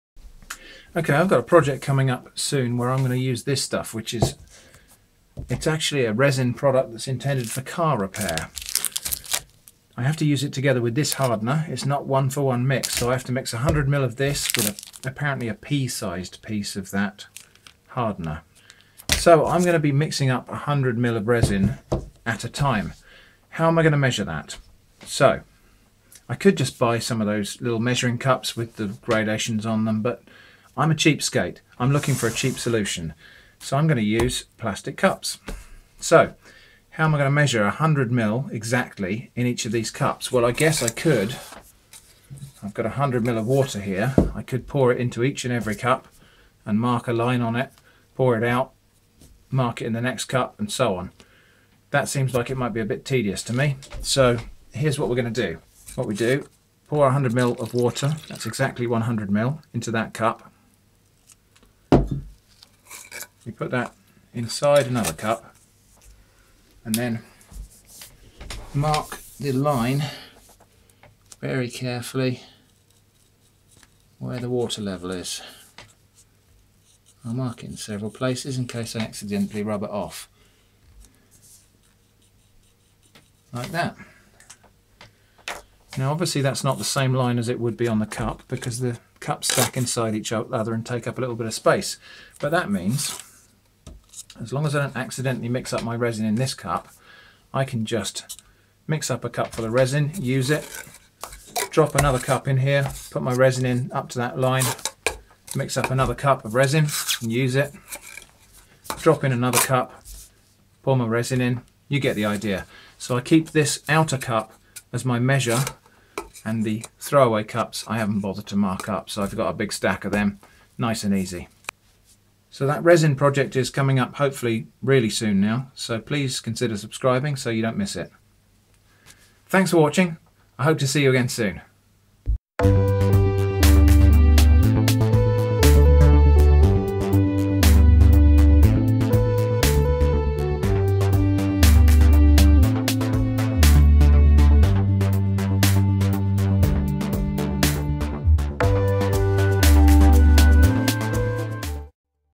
Quick Tip For Measuring Resin Accurately With Cheap Disposable Cups
I've got a project coming up soon where I need to mix up a series of 100ml batches of resin - and I don't feel like spending out on a load of measuing beakers.

So here's a really quick tip on how to measure accurate and consistent amounts of resin using only very cheap disposable plastic drinking cups.